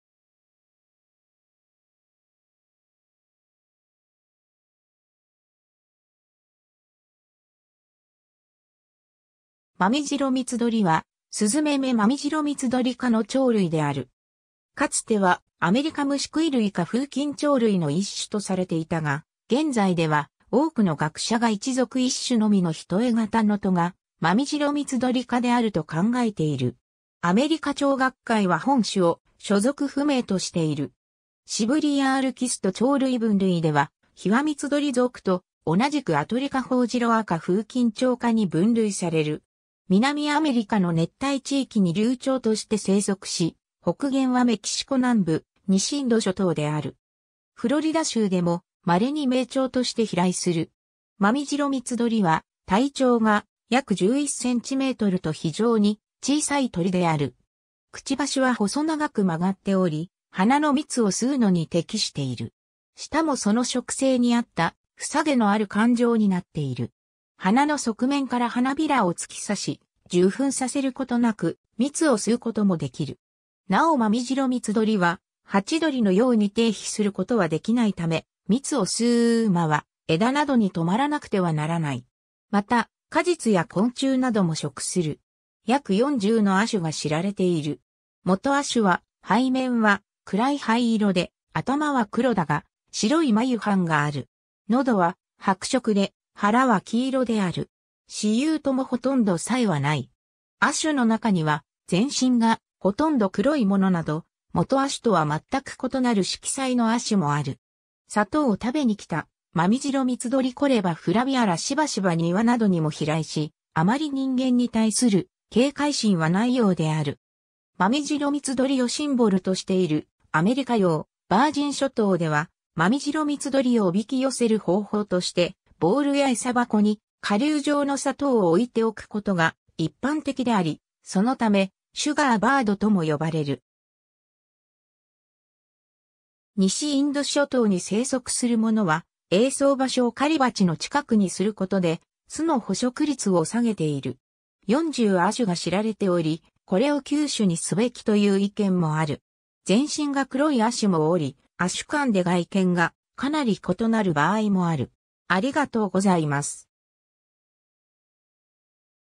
マミジロミツドリ
マミジロミツドリ（学名：Coereba flaveola）は、スズメ目マミジロミツドリ科の鳥類である。
 かつてはアメリカムシクイ類かフウキンチョウ類の一種とされていたが、現在では多くの学者が1属1種のみの単型の科「マミジロミツドリ科」であると考えている。
アメリカ鳥学会(AOU) は本種を所属不明としている。
シブリー＝アールキスト鳥類分類では、ヒワミツドリ属と同じくアトリ科ホオジロ亜科フウキンチョウ科に分類される。
 南アメリカの熱帯地域に留鳥として生息し、北限はメキシコ南部、西インド諸島である。
フロリダ州でも稀に迷鳥として飛来する。
 マミジロミツドリは体長が約11センチメートルと非常に小さい鳥である。
くちばしは細長く曲がっており、花の蜜を吸うのに適している。
舌もその食性に合ったふさ毛のある管状になっている。
花の側面から花弁を突き刺し、受粉させることなく蜜を吸うこともできる。
なおマミジロミツドリはハチドリのように停飛（ホバリング）することは出来ないため、蜜を吸う間は枝などに止まらなくてはならない。
また、果実や昆虫なども食する。
 約40の亜種が知られている。
基亜種は背面は暗い灰色で、頭は黒だが白い眉斑がある。
のどは白色で、腹は黄色である。
雌雄ともほとんど差異はない。
亜種のなかには、全身がほとんど黒いものなど、基亜種とは全く異なる色彩の亜種もある。
 砂糖を食べに来たマミジロミツドリ Coereba flaveola しばしば庭などにも飛来し、あまり人間に対する警戒心はないようである。
マミジロミツドリをシンボルとしているアメリカ領ヴァージン諸島では、マミジロミツドリを誘き寄せる方法としてボウルや餌箱に顆粒状の砂糖を置いておくことが一般的であり、そのため "Sugar bird" とも呼ばれる。
（ヒワミツドリ属も同様） 西インド諸島に生息するものは、営巣場所を狩りバチの近くにすることで、巣の捕食率を下げている。
 40亜種が知られており、これを9種にすべきという意見もある。
全身が黒い亜種もおり、亜種間で外見がかなり異なる場合もある。